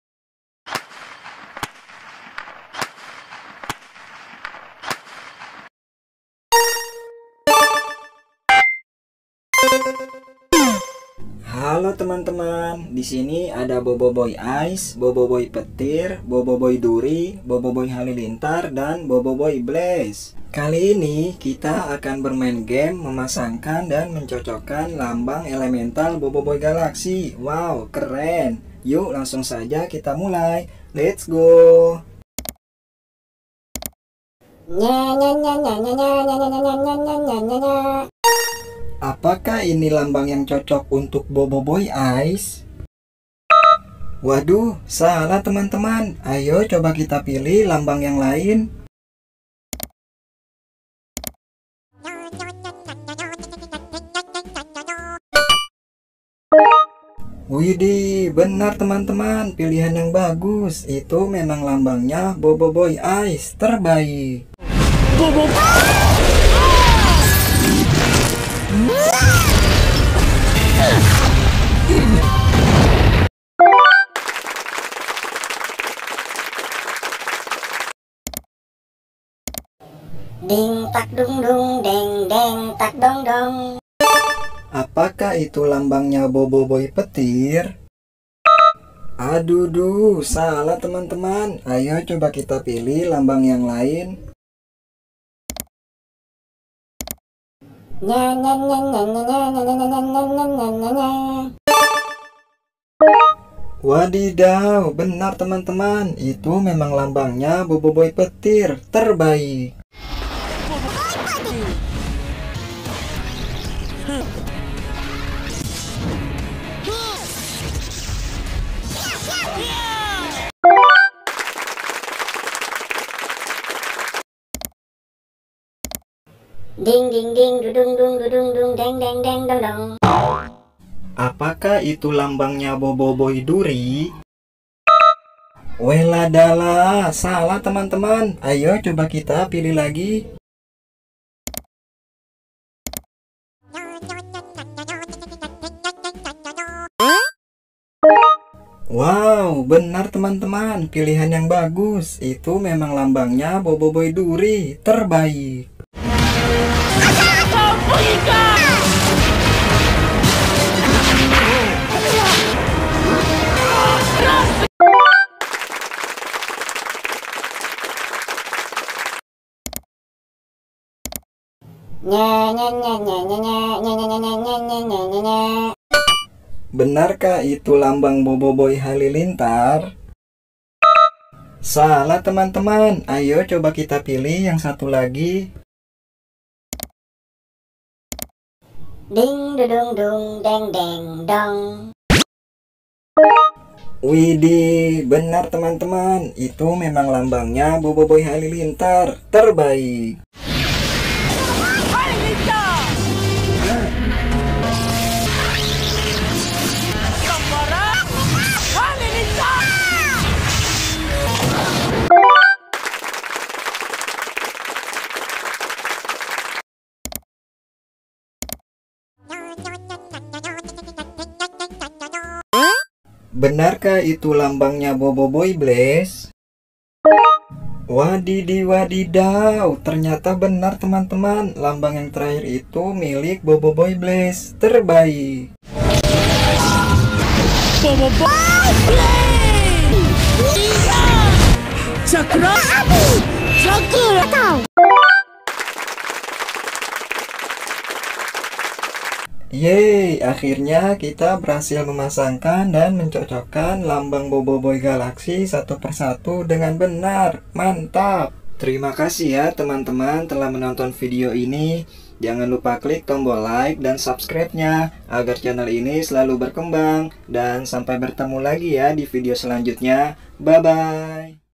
MEMASANGKAN LAMBANG KUASA BOBOIBOY GALAXY | BOBOIBOY HALILINTAR, BOBOIBOY AIS, BOBOIBOY BLAZE, PETIR
Bermain game,, memasangkan / mencocokkan 5 lambang kuasa elemental Boboiboy galaxy dengan benar.
Boboiboy ais, Boboiboy halilintar, Boboiboy blaze, Boboiboy petir, Boboiboy duri